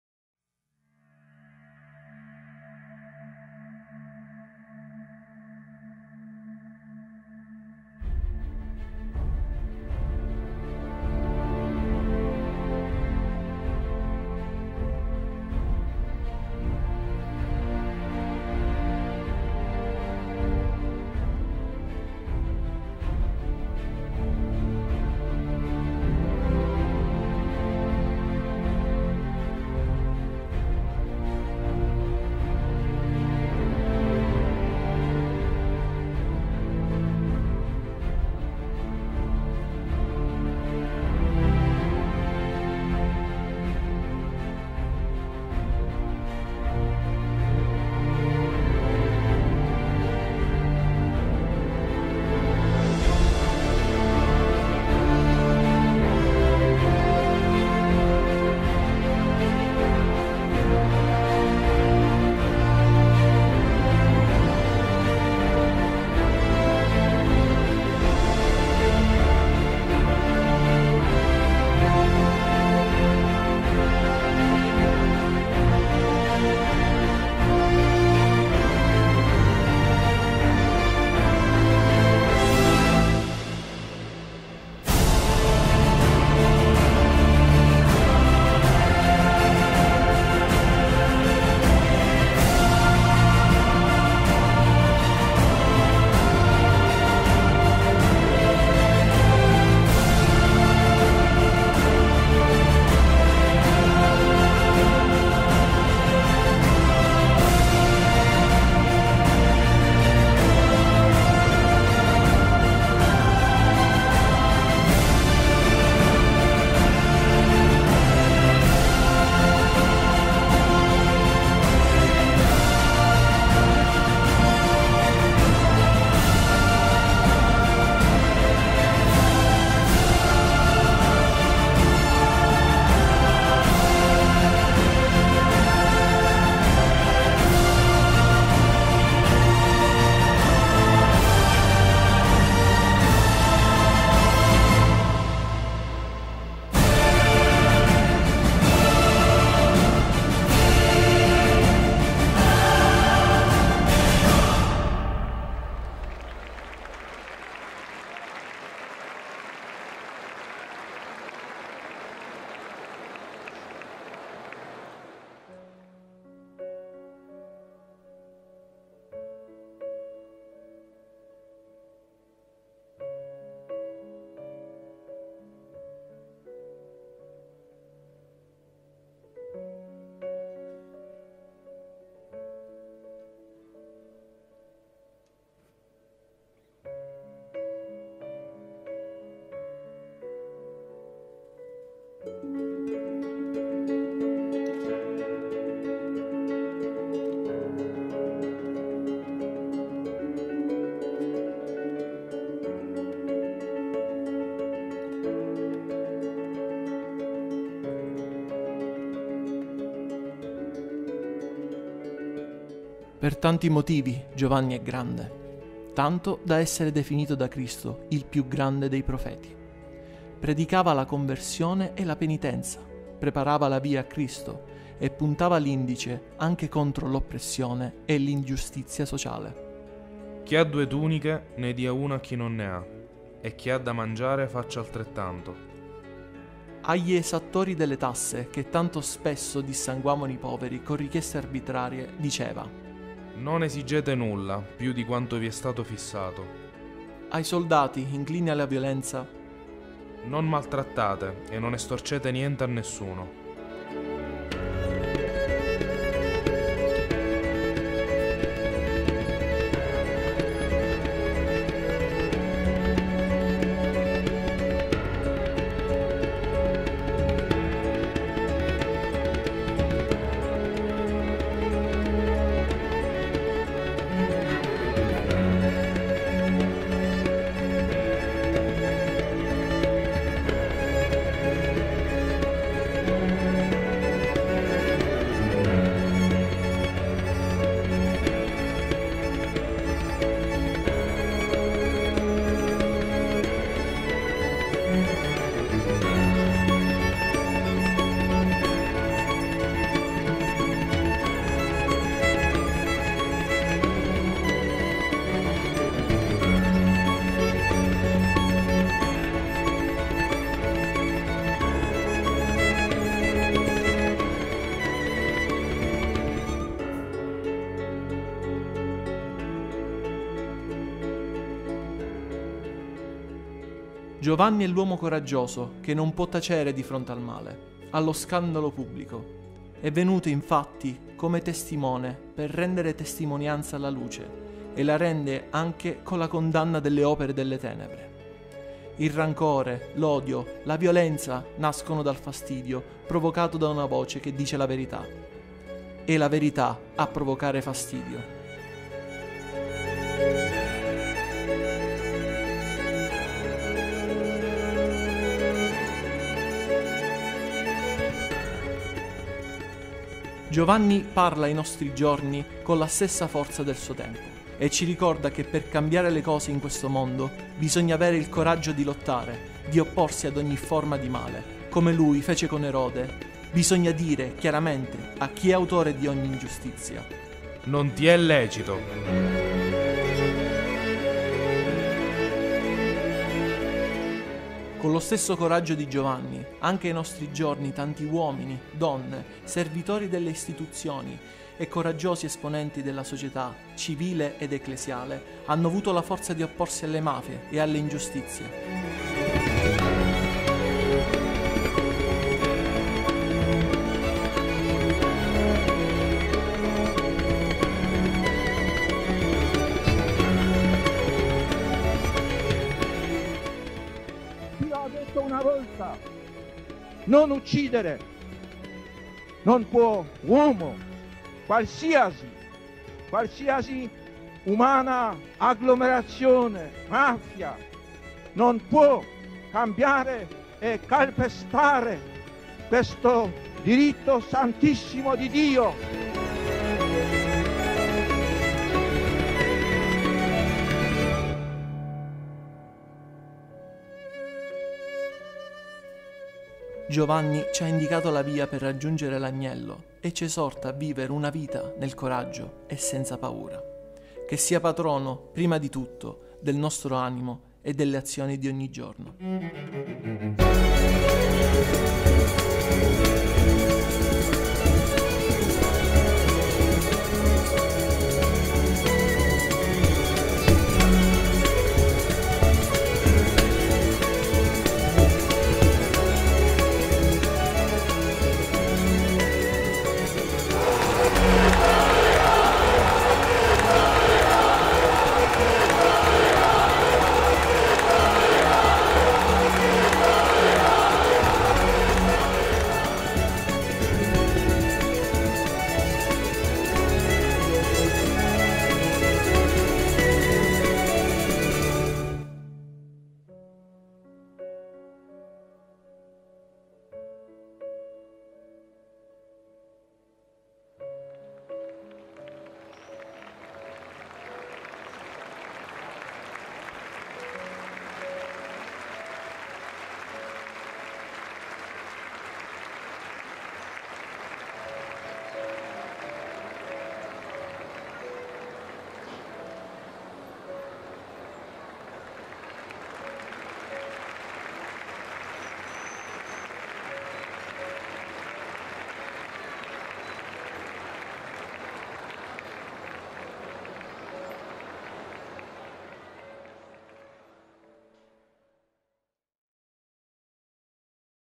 Video Mapping | San Giovanni Battista | Ragusa 2019
Video Mapping realizzato in occasione dei festeggiamenti in onore di San Giovanni Battista, Patrono della città e della diocesi di Ragusa giorno 29 Agosto 2019.